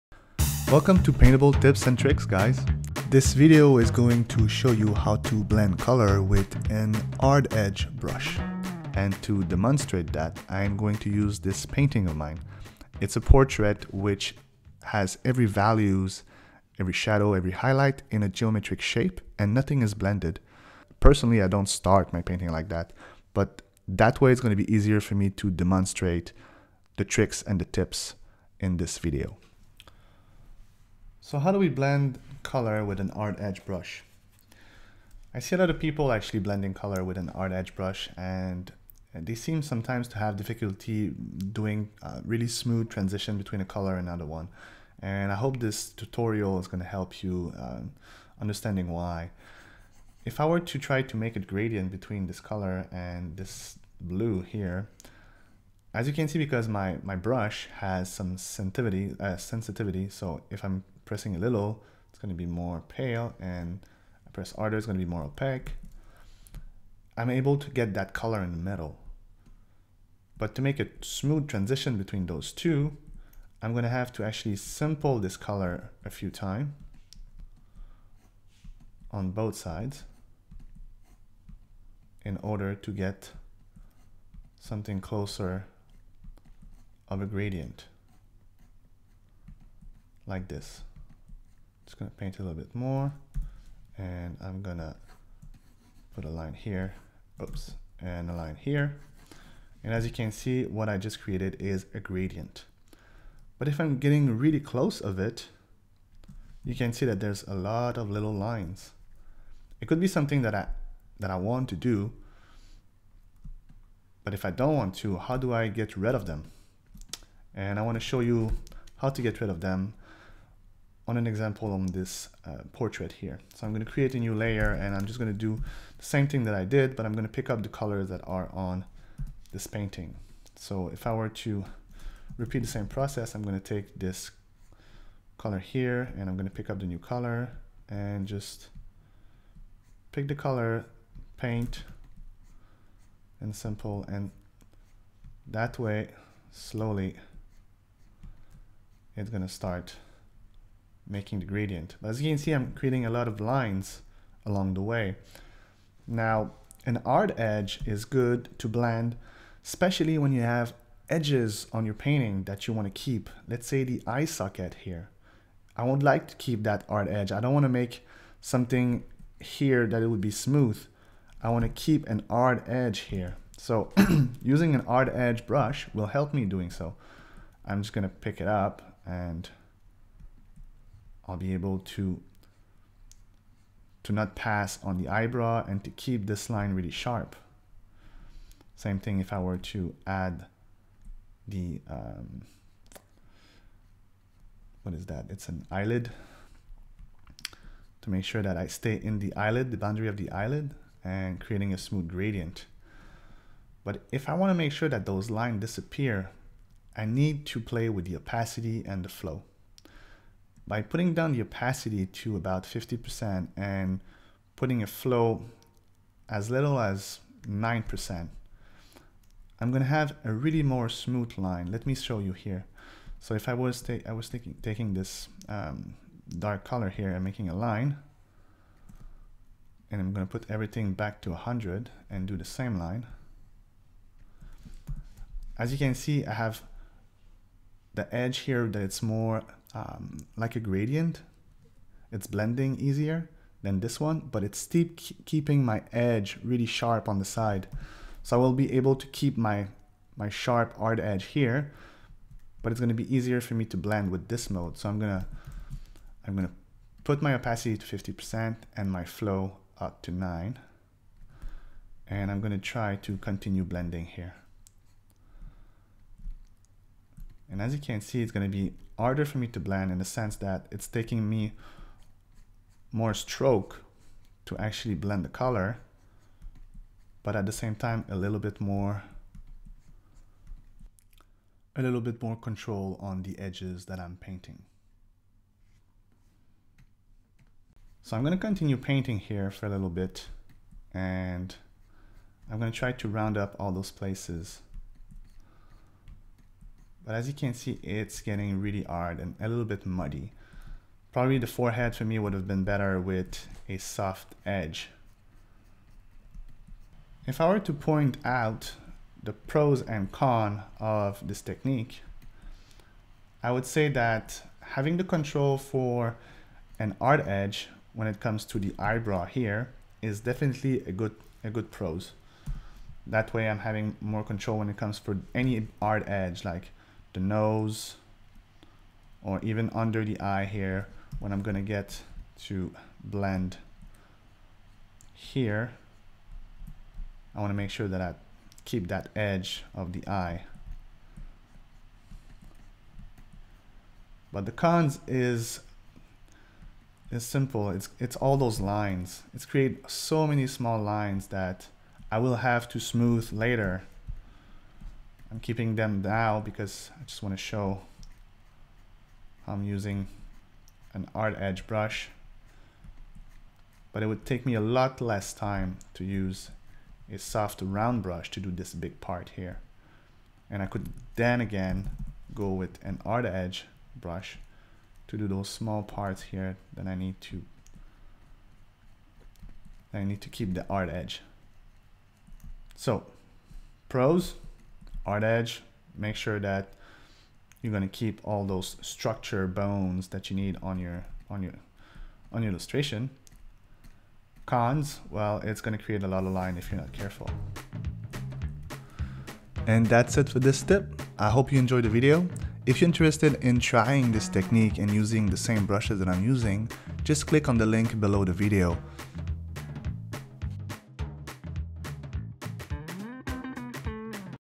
How to Blend Colors Using Photoshop's Hard Edge Brush
Welcome to our Tips & Tricks video series here on Paintable. In this video, David shows in detail how you can use the hard edge brush to blend colors in Photoshop.

Learn now how you can blend colors in Adobe Photoshop using the hard edge brush.

👉 To learn more about digital painting, join the Digital Painting Academy

Check out these other videos for more tips and tricks on blending colors with Photoshop: